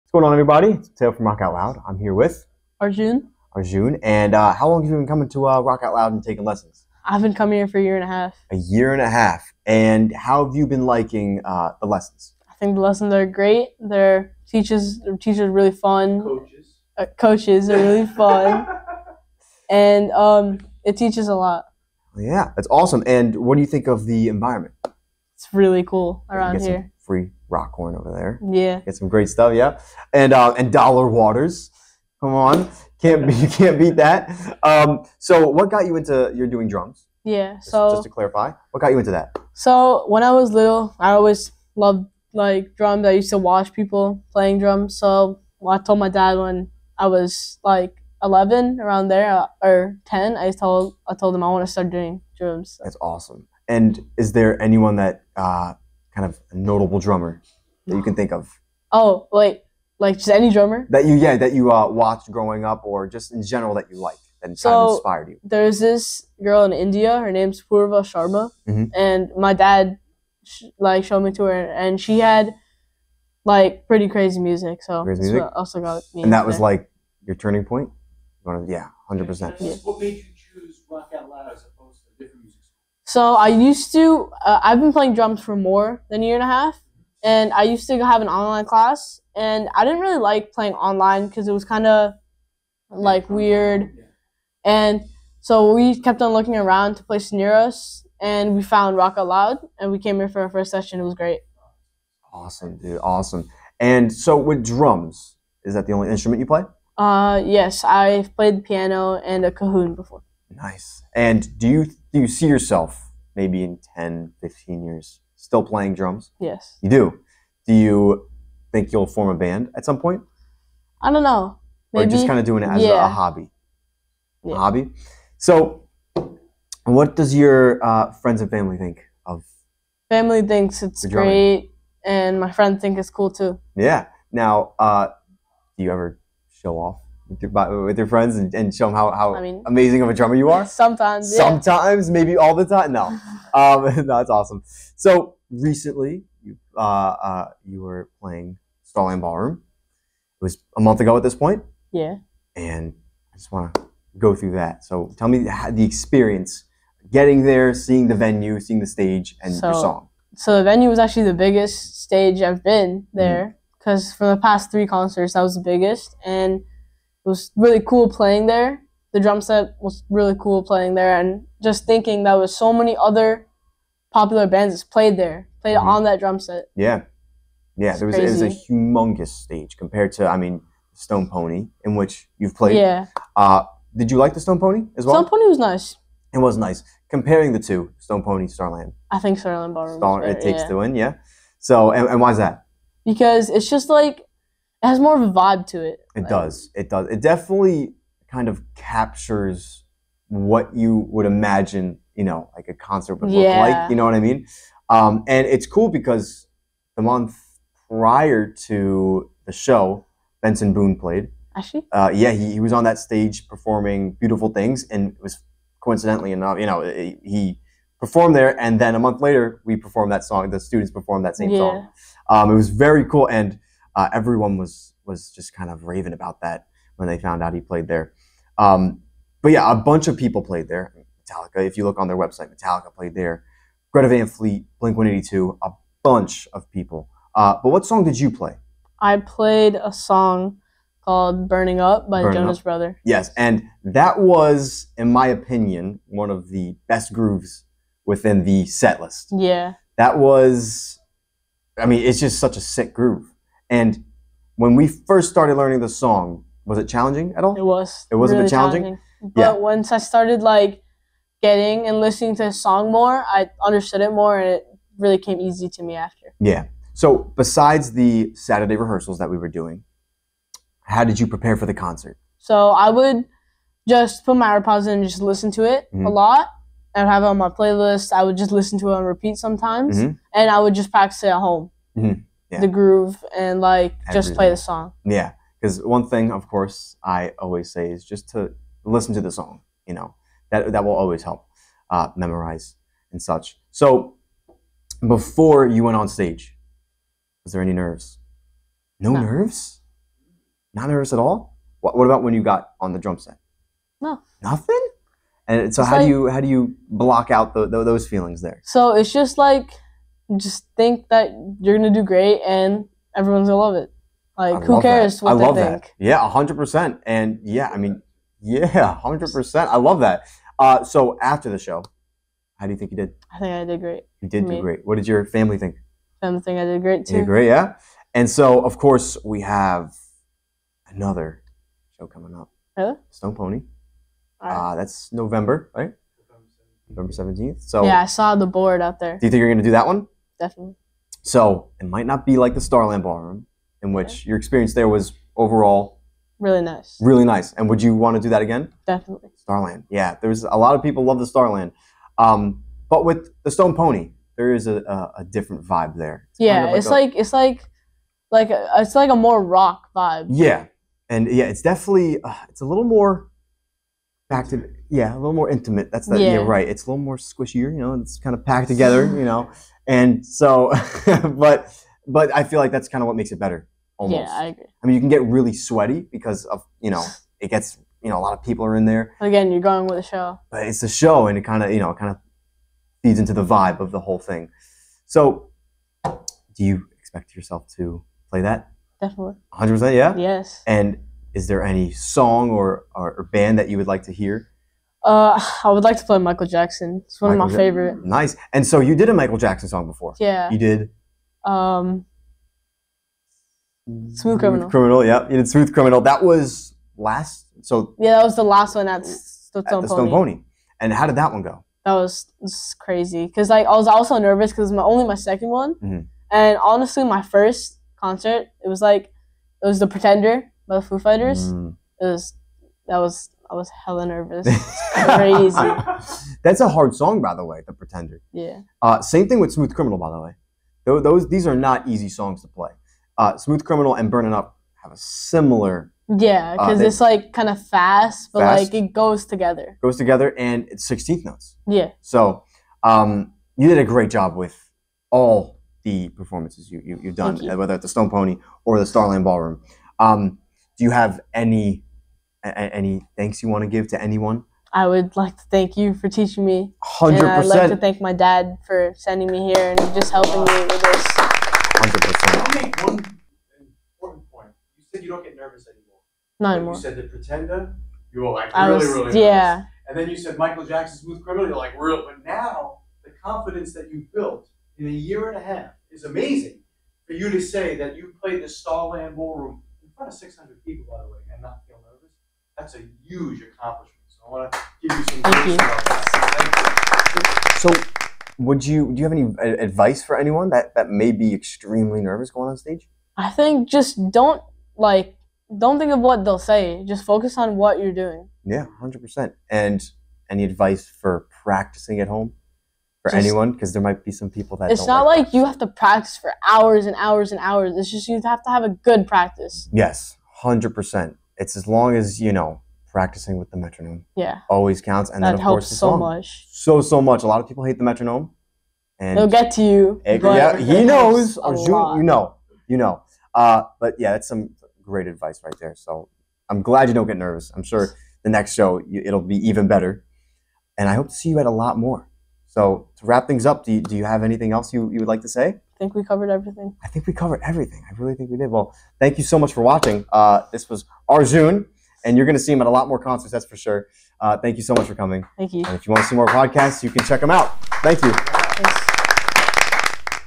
Arjun talks all about being a drummer!
Arjun recently played the Starland Ballroom on June 30 to over 400 people! How many people can say that?
Here is an interview Arjun has with his Coach and he speaks all about the experience!